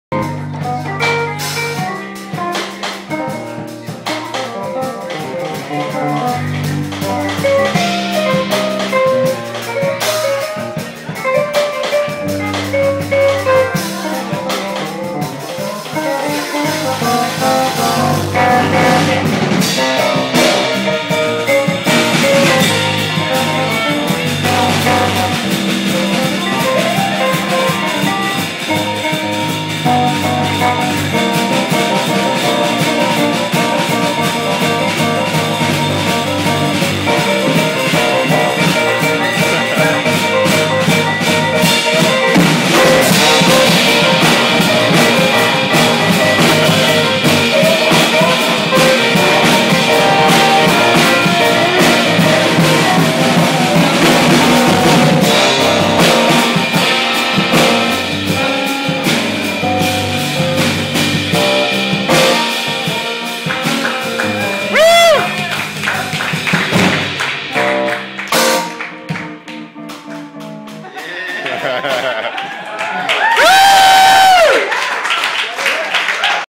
EWU Jazz Collective at the Mason Jar
Sorry for the poor quality - this was filmed on an iPhone.  Garrett Stannard, Lauren McKinley, Chris Shepart, and Jake Madison at the Mason Jar in Cheney, WA.  Special guest appearance - Riley Gray on the Piano.